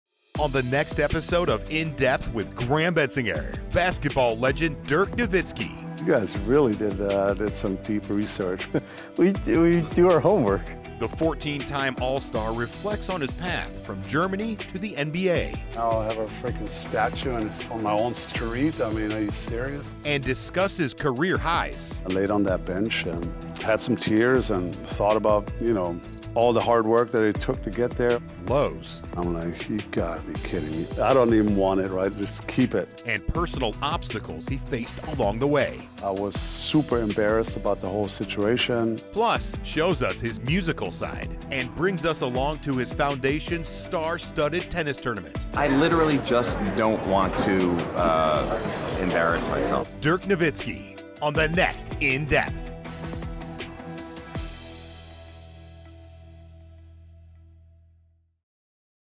Dirk Nowitzki: Feature Episode Preview
Everything is bigger in Texas, including the welcoming personality of 7-foot-tall Dallas Mavericks legend, Dirk Nowitzki. We spent two days with the former NBA MVP in the city he set down roots in throughout his entire 21-year NBA career. Dirk reflects on the quirky German who primed him for the League, regrets after a rough rookie season and the arrest of a fraudulent former fiancée. Plus, we get a taste of his musical talents and drop by the fifth-annual Dirk Nowitzki Pro Celebrity Tennis Classic, where we hear from Steve Nash and Ben Stiller, among others.